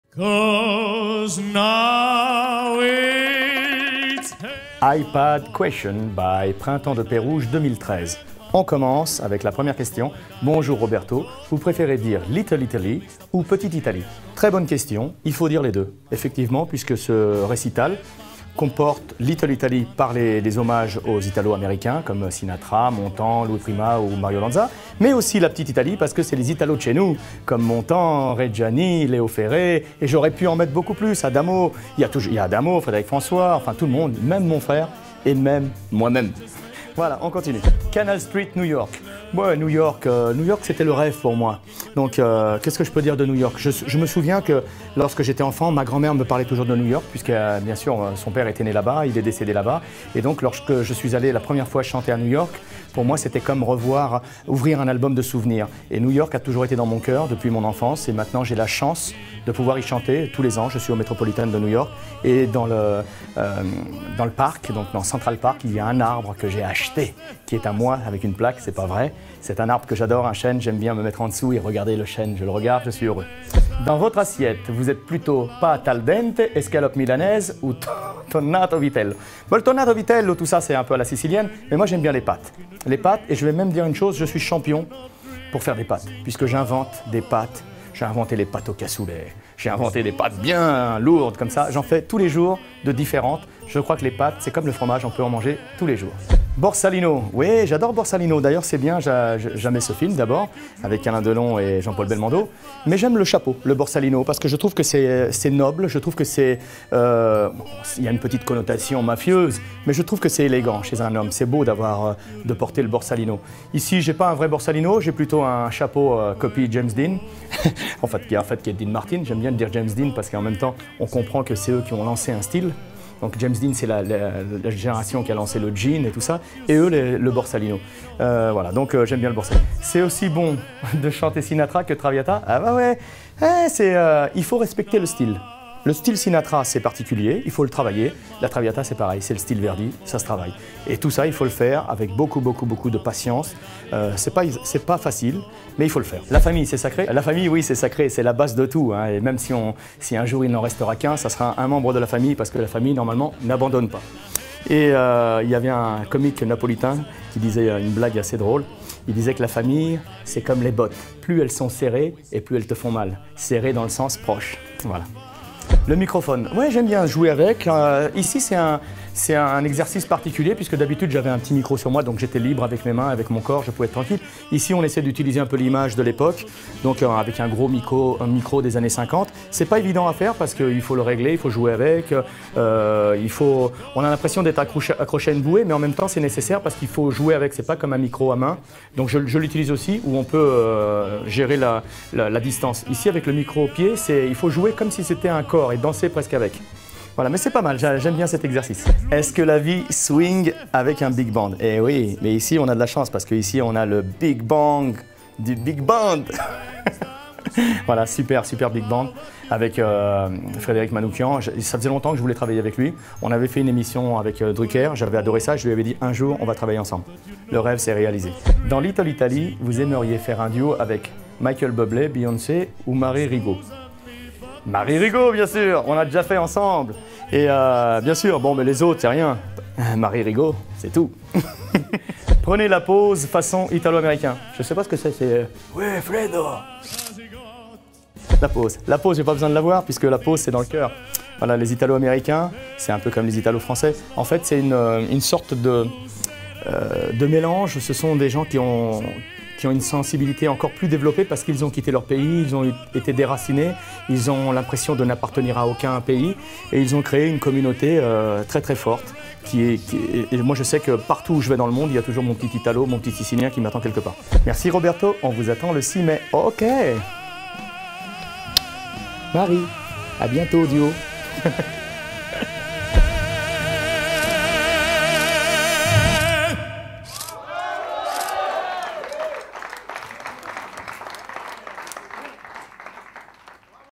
Apple Ipad Interview Roberto ALAGNA (official !)
Roberto ALAGNA en concert le 6 mai 2013 à la Cité internationale de Lyon dans le cadre du Printemps de Pérouges.